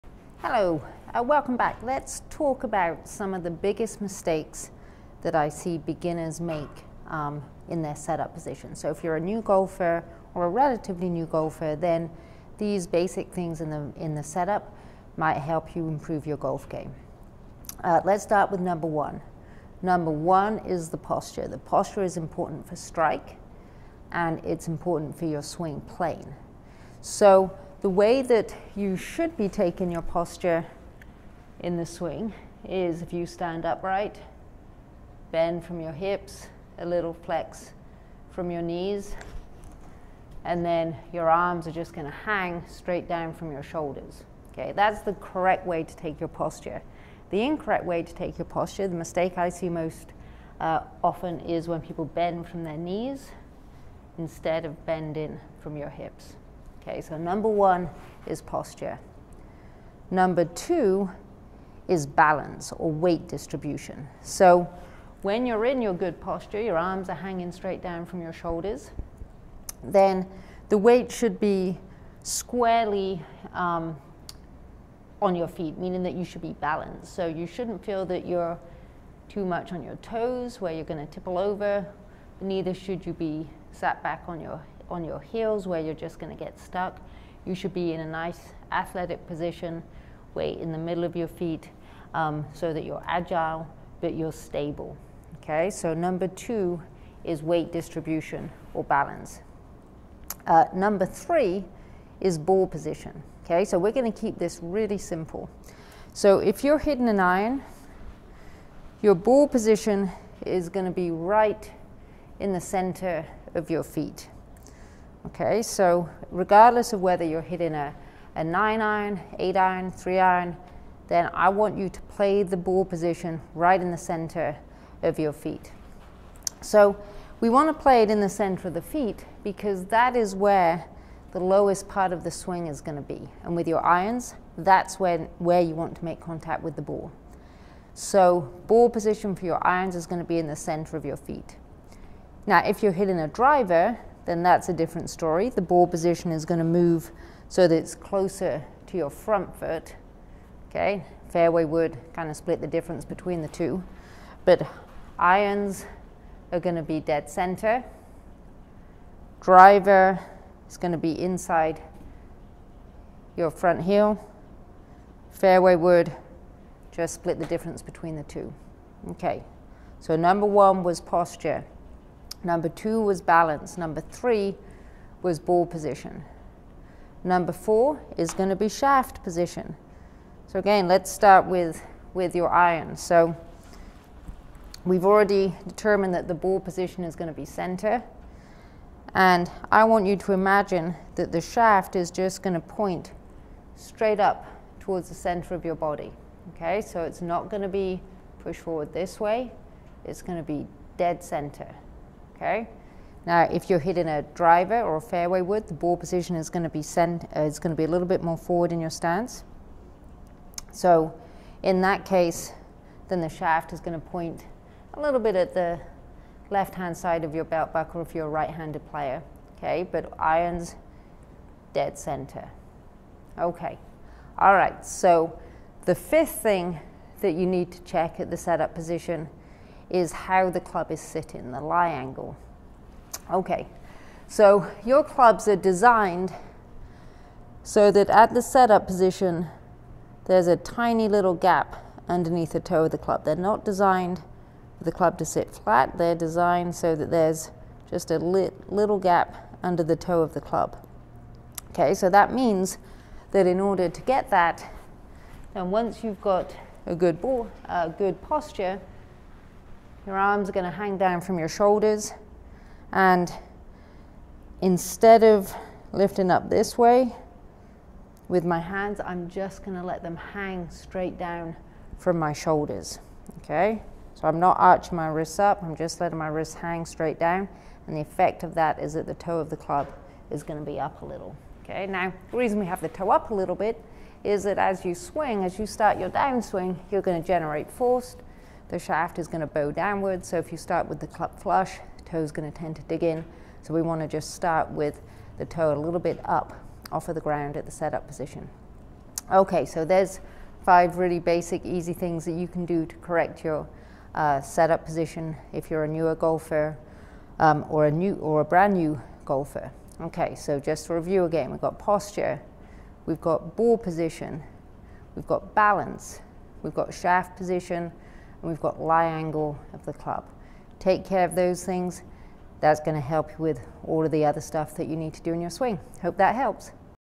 The 5 biggest set-up mistakes golfers make
Mistakes in your set-up can cause swing problems. This video goes over the  5 biggest set-up mistakes golfers make. Fixing these mistakes will help you build a better swing and improve your golf game.

Sue Shapcott, PhD, PGA GB&I
Change Golf Instruction, Madison